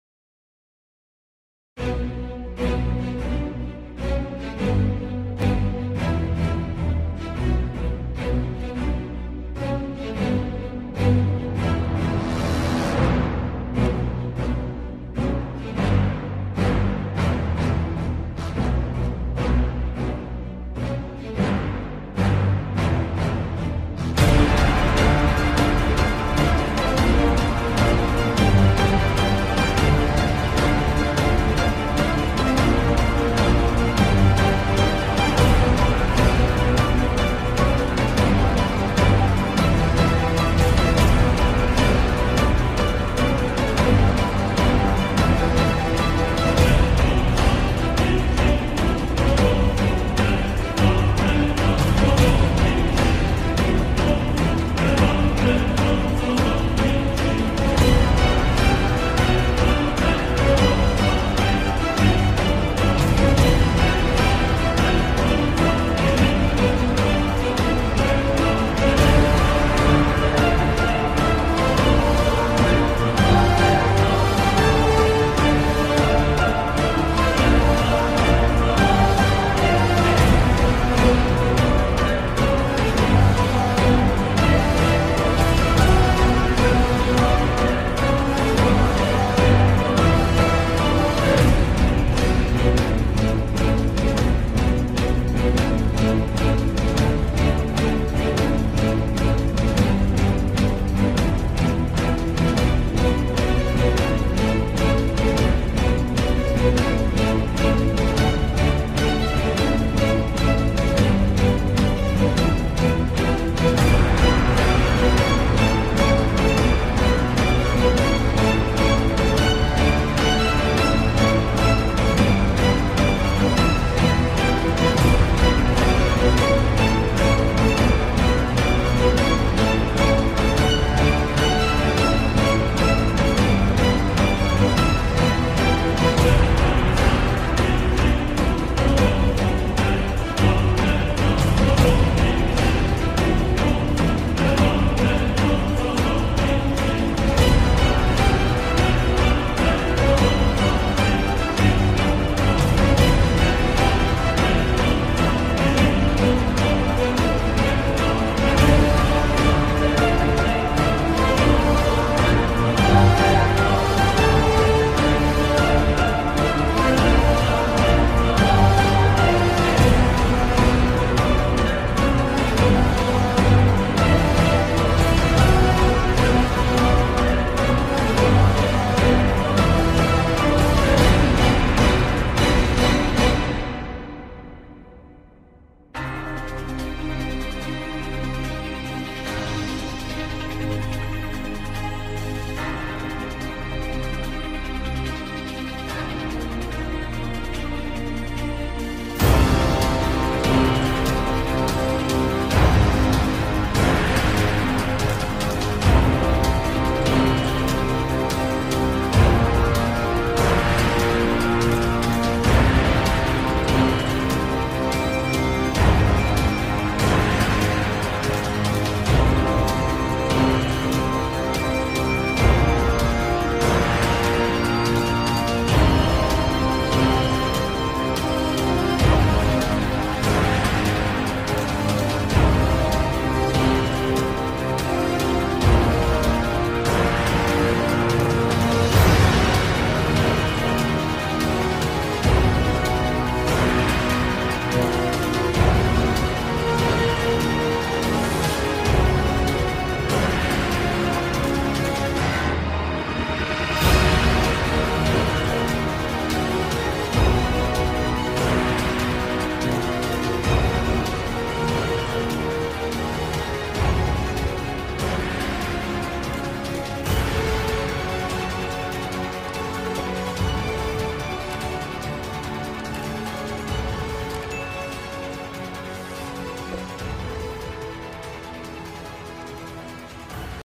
IRAN vs UAE | military power comparison 2021 - 2022
Iran vs United Arab Emirates military power comparison by side by format. I compare those two countries Land, Air, and Naval power. Iran and UAE two neighbour rival country and they have strong military power. @MILITARY POWER COMPARISON

Ansia Orchestra

Source Video [MFY - No Copyright